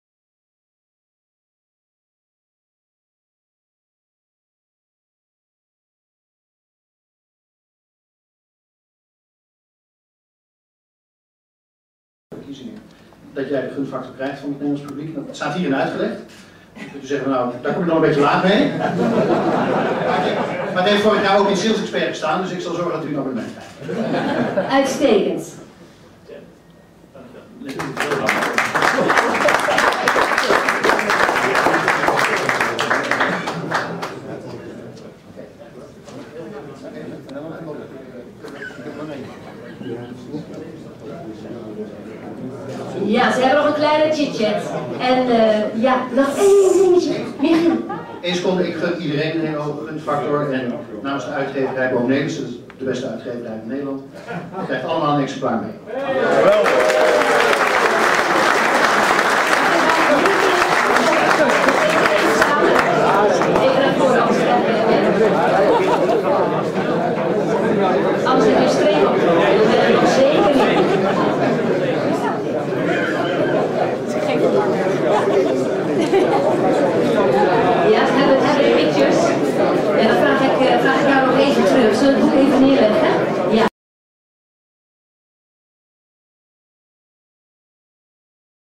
Presentatie 'Verhoog je Gunfactor' aan Jan Peter Balkenende
Op 20 oktober 2011 nam Jan Peter Balkenende tijdens een feestelijke Gala avond het eerste exemplaar van 'Verhoog je Gunfactor' in ontvangst.